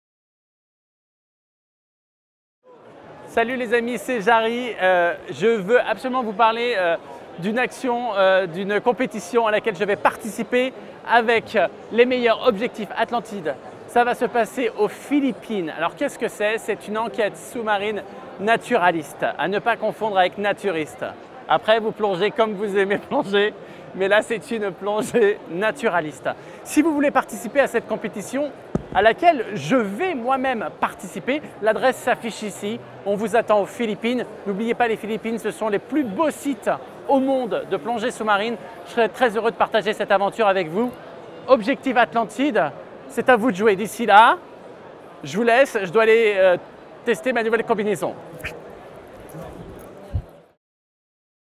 Jarry Parrain d'Objectif Atlantide aux Philippines
Participation de Jarry à l'enquête naturaliste d'Objectif Atlantide aux Philippines à Padre Burgos, île de Leyte.